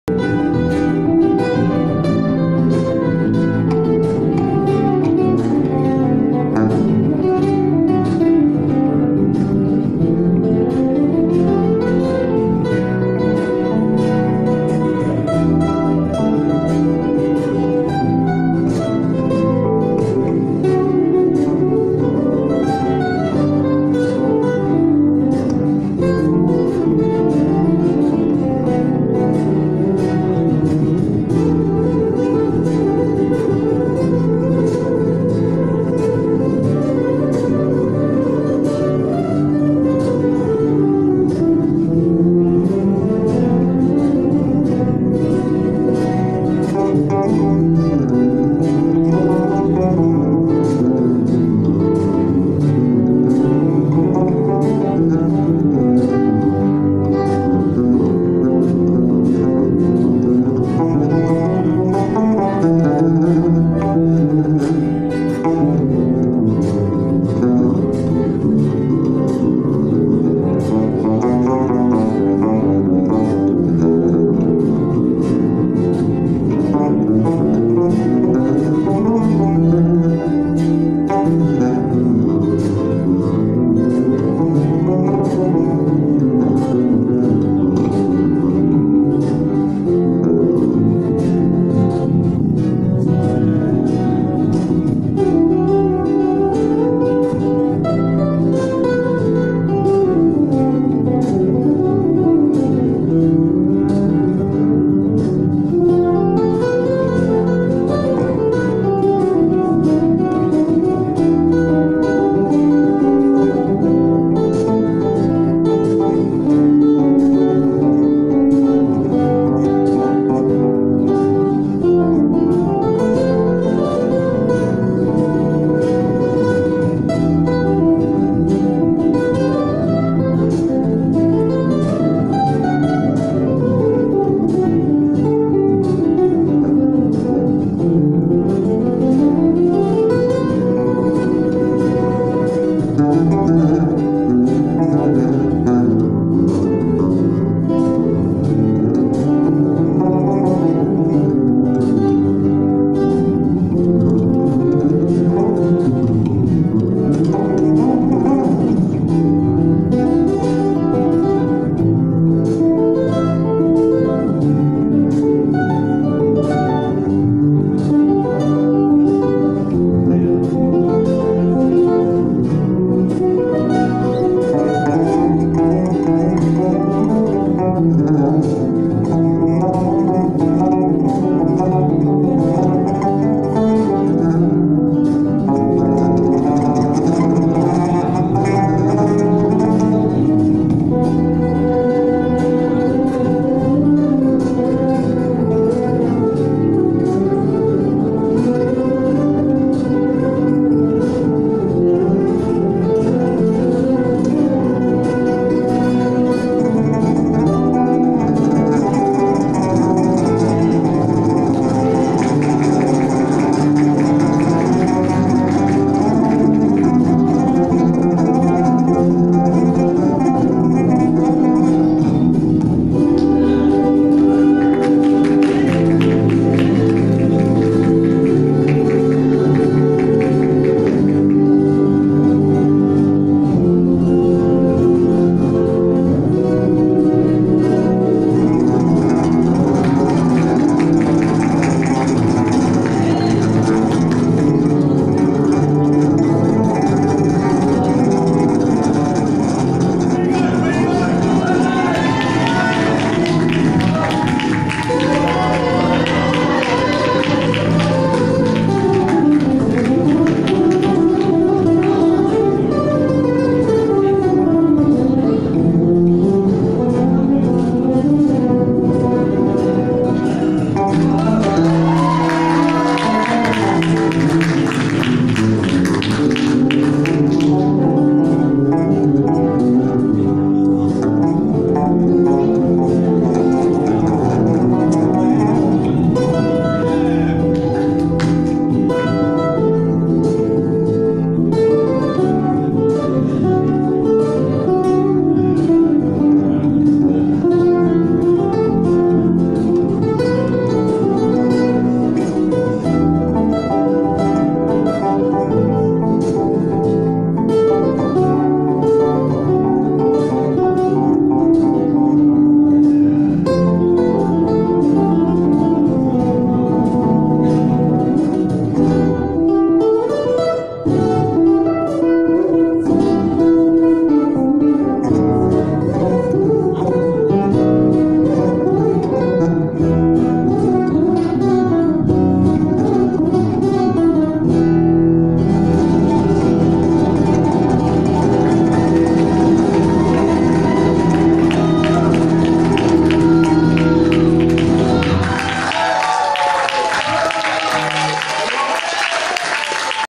Bernardo Soler and Dean Gaudoin live @ GEMCO Theatre Emerald, Victoria Australia
Bernardo Soler - uses Godin guitar ACS Slim nylon string
Dean Gaudoin - uses ESP basses
Camera - Samsung Galaxy S7 mobile phone